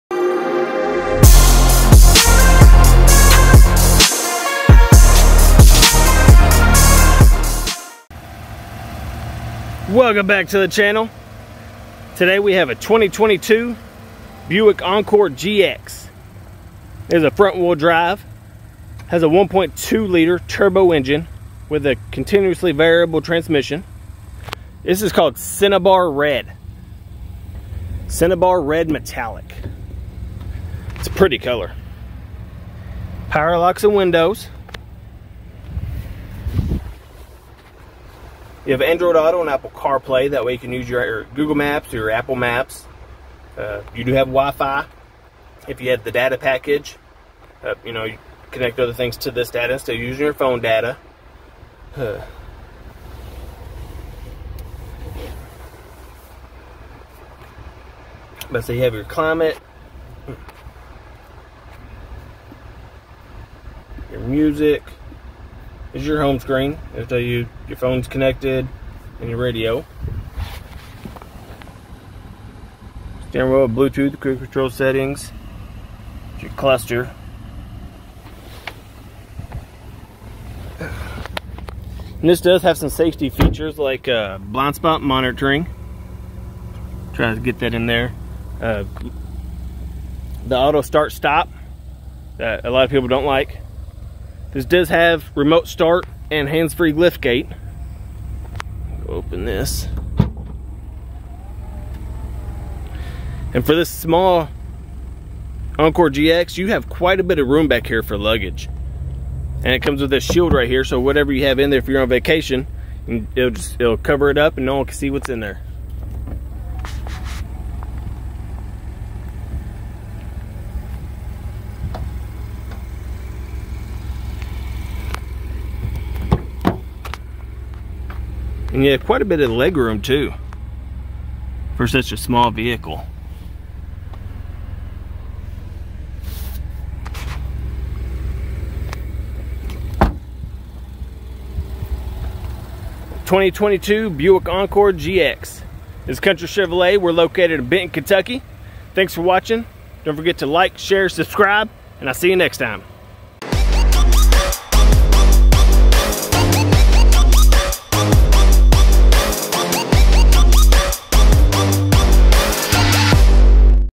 2022 Buick Encore GX (STK: 60259) Walk Around
2022 Buick Encore GX Select FWD
STK: 60259
CVT
ECOTEC 1.2L Turbo
29/31 City/Highway MPG
4D Sport Utility
Cinnabar Metallic
Ebony With Ebony Interior Accents Cloth

Your Local GM dealer! Experts in Chevrolet, Buick, and GMC!

Country Chevrolet Buick GMC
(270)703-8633